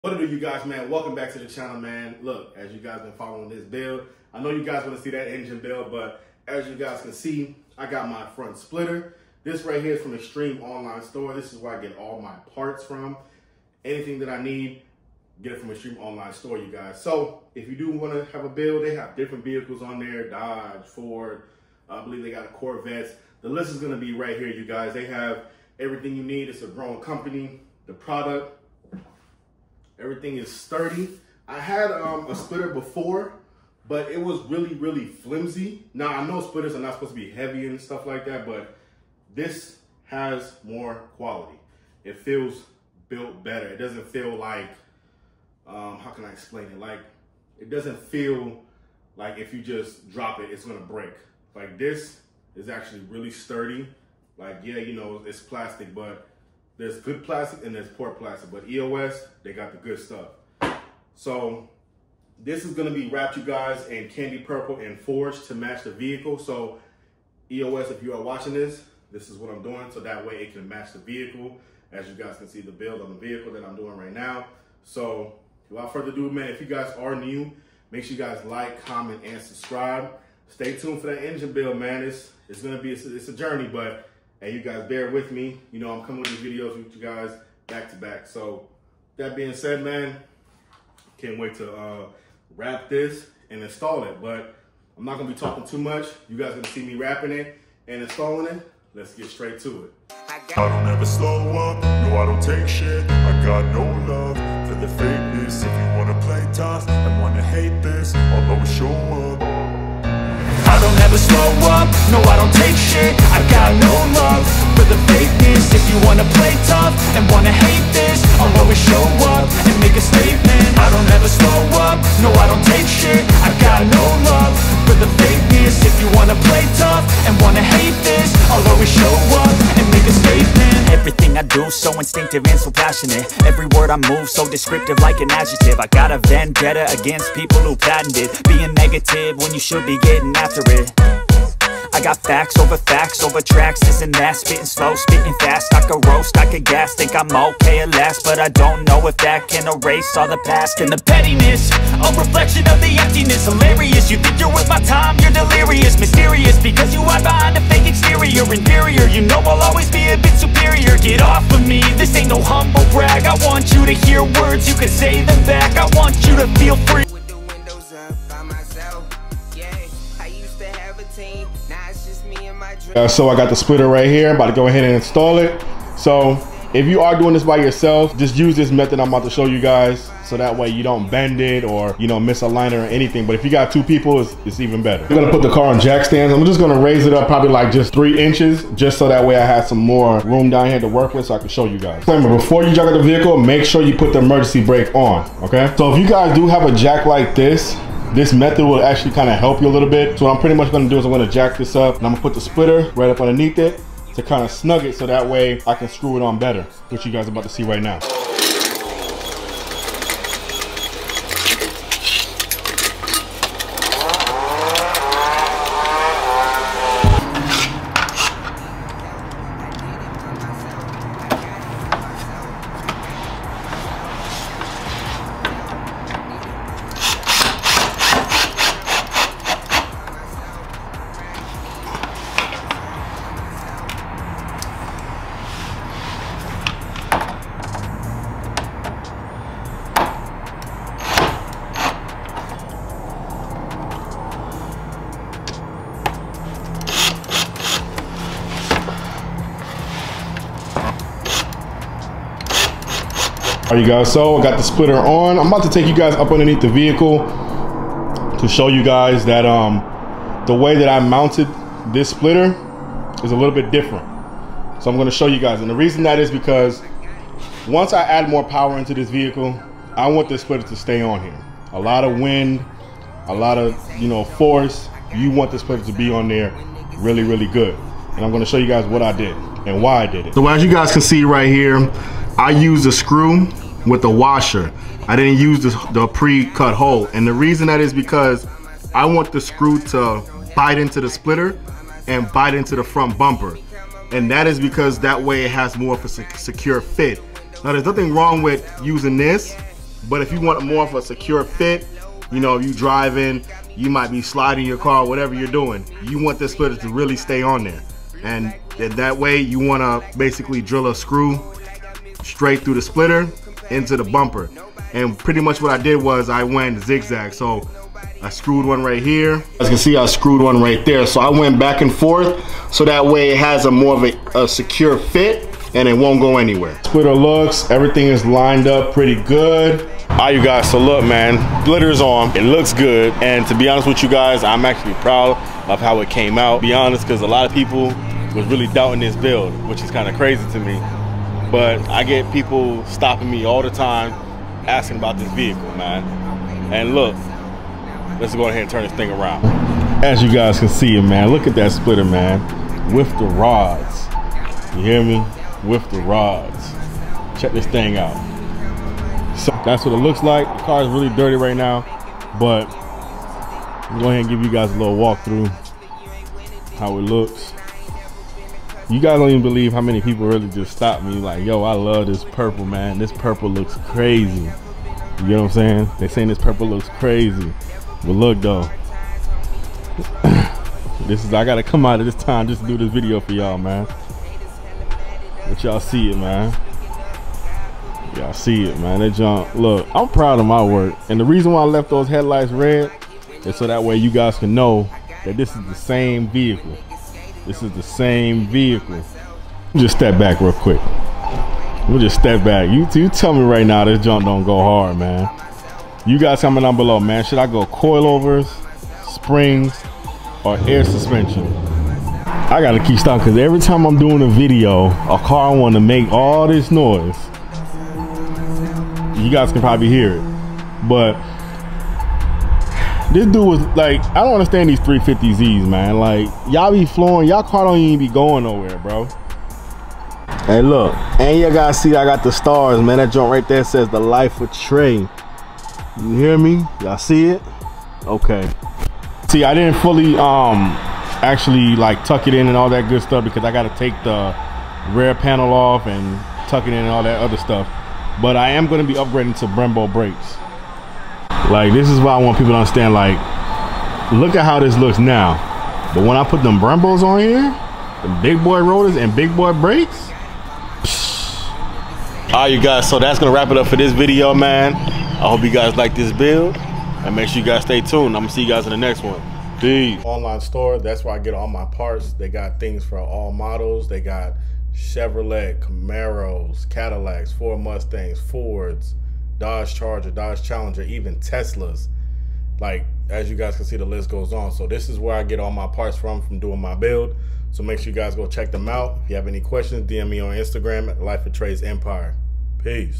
Transforming My Dodge Charger With An Extreme Online Store Splitter Installation! *MUST WATCH*
➡️Comment Some Ideas You Would Like To See….